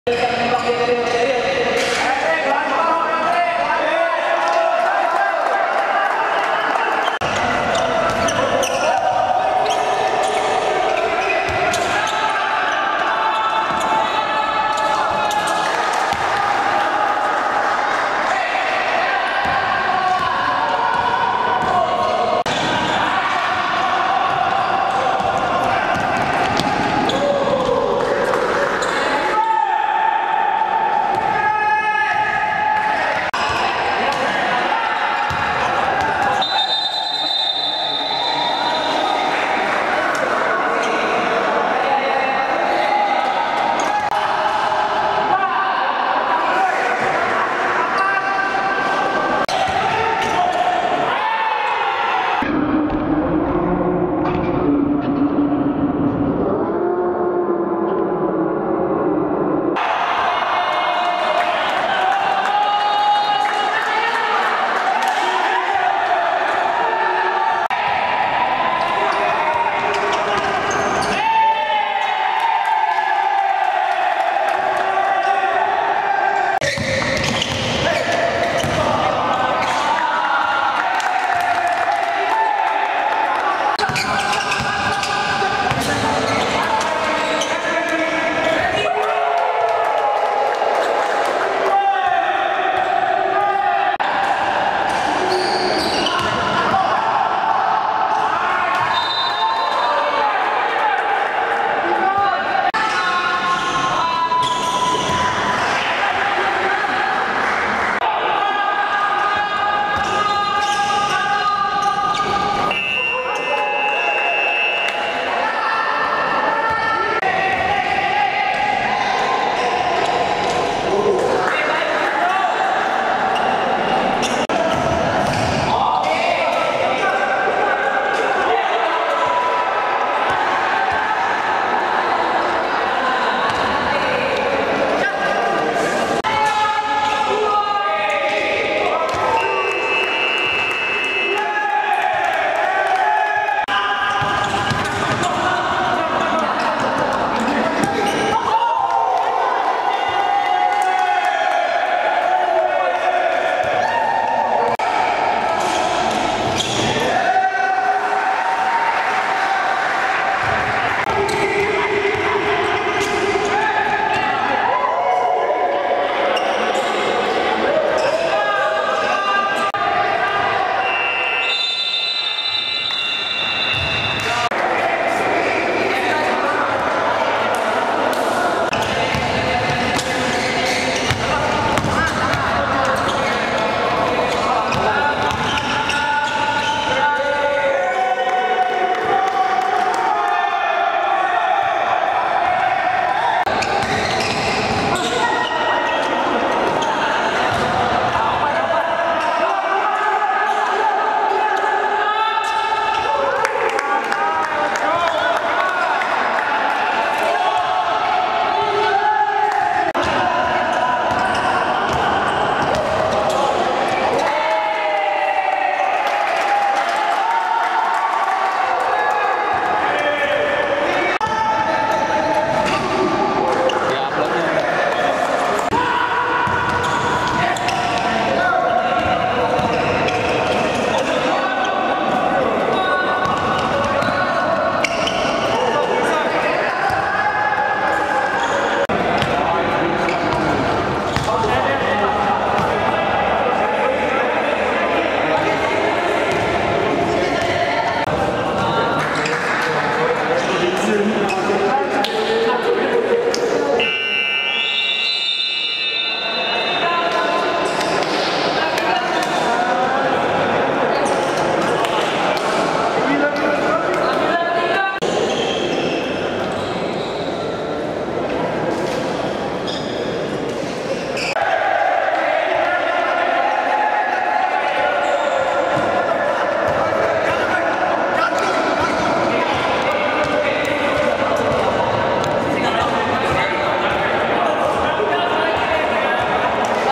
BDD Highlights | PON DKI Jakarta vs NSH MGT - Scrimmage. Ali Bagir berapa kali dunk?
2 September 2021
Scrimmage
PON DKI Jakarta vs NSH Mountain Gold Timika
Highlights scrimmage kali ini diambil dari quarter ke 3 dan ke 4.
Menyongsong PON XX PAPUA 2021 yang akan dimulai pada bulan Oktober, tim-tim basket PON mulai menjalani uji coba. Uji coba / scrimmage kali ini melawan tim profesional IBL, yaitu NSH Mountain Gold Timika (belum full team).

CHEERS!